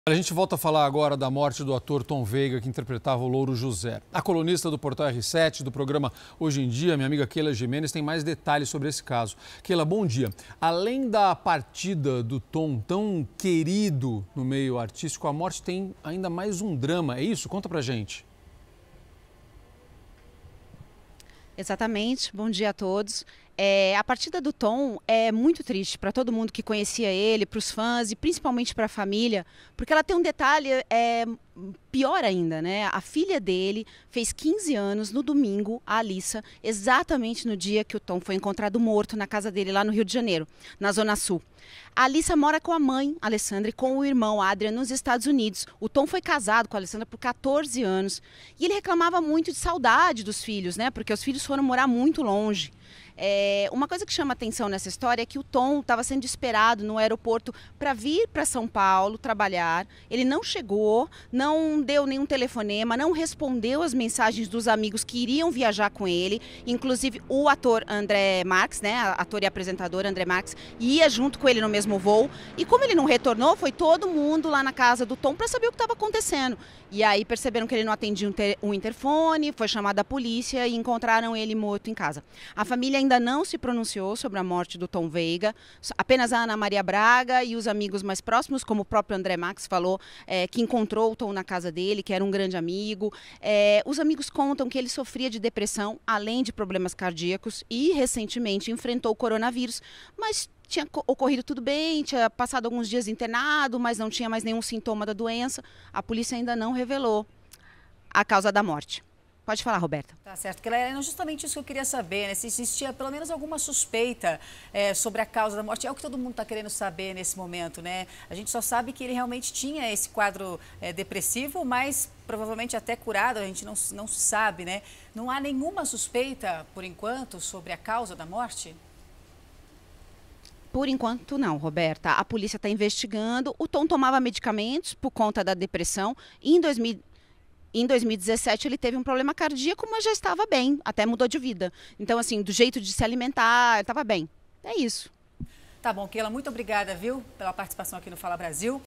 Intérprete do Louro José morre no dia do aniversário da filha
O intérprete do Louro José foi encontrado morto neste domingo (2), no mesmo dia em que a filha dele completou 15 anos. Amigos também relatam que Tom Veiga reclamava da saudade dos filhos, que moravam longe, e sofria de depressão.